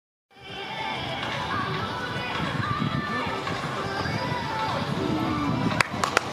Jankovics Dániel harmadik ugrása a 2.24 m-en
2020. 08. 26. Székesfehérvár, Magyar Egyetemi és Főiskolás Országos Bajnokság